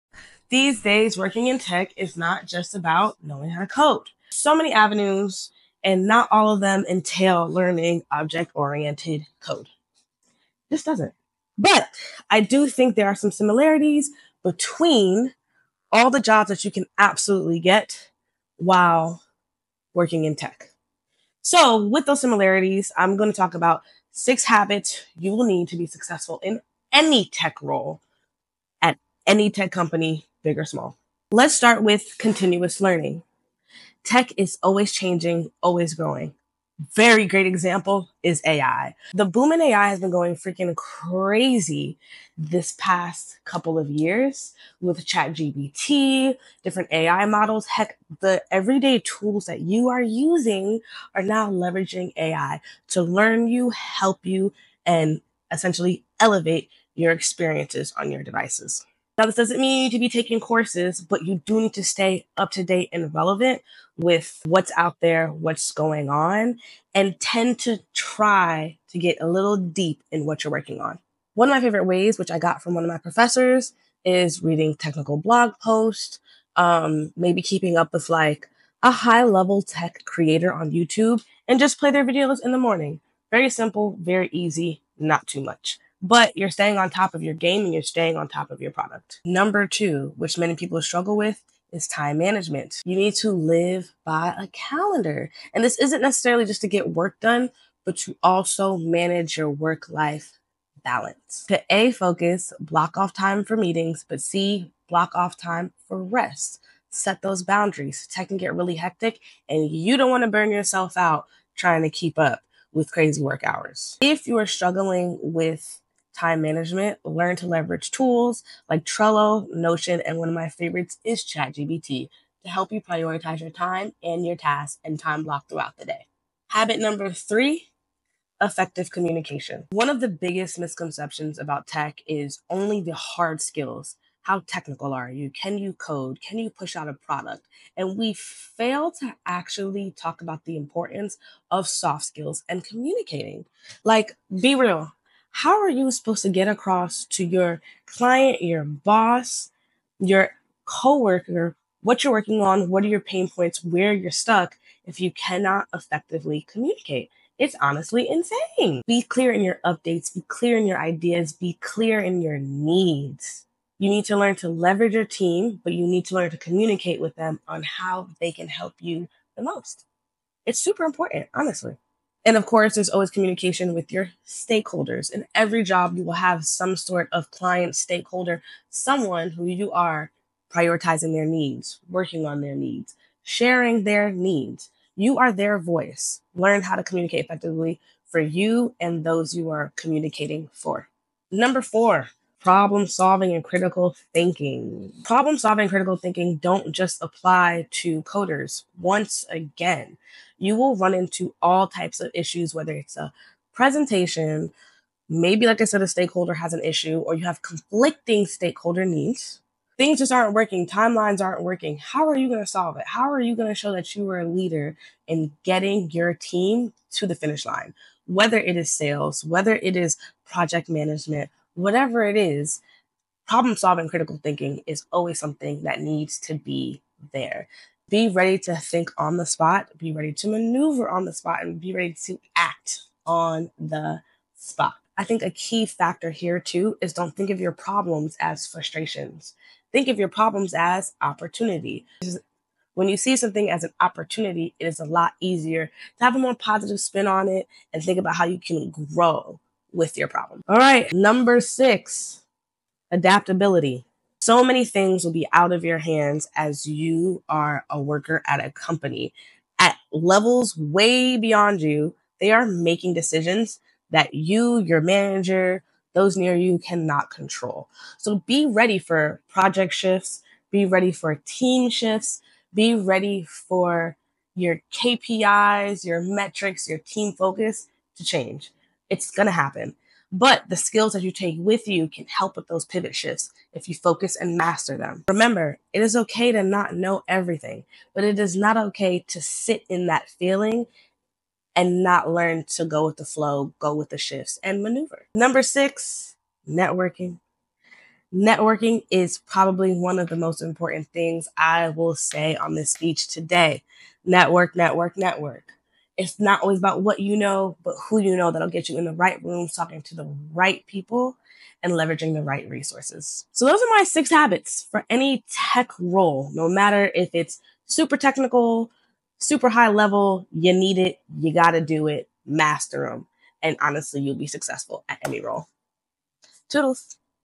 Find Success With A Tech job
Curious about how to be successful working in tech, no matter your role? In this video, I share six essential habits that will help you thrive in any tech position, whether you’re working at a startup or a major tech company. From continuous learning and time management to the importance of communication and critical thinking, these habits are crucial for anyone looking to make an impact. Whether you're a beginner or a seasoned professional, this video offers valuable insights to help you master soft skills, adapt to changes, and excel in any tech role. Join me as I discuss how these habits can transform your career in tech!

Timestamps:
0:00 - Introduction to Succeeding in Tech Roles
0:31 - Habit 1: Continuous Learning in a Fast-Changing Industry
1:17 - Habit 2: Mastering Time Management for Work-Life Balance
2:02 - Habit 3: Effective Communication in Tech
3:09 - Habit 4: Problem Solving & Critical Thinking
4:13 - Habit 5: The Importance of Adaptability in Tech
5:00 - Habit 6: Building a Strong Network in the Tech Industry
5:47 - Final Thoughts: Master These Habits for Career Success

✨Please✨ * * * * * * *
● 🌐 Amazon Storefront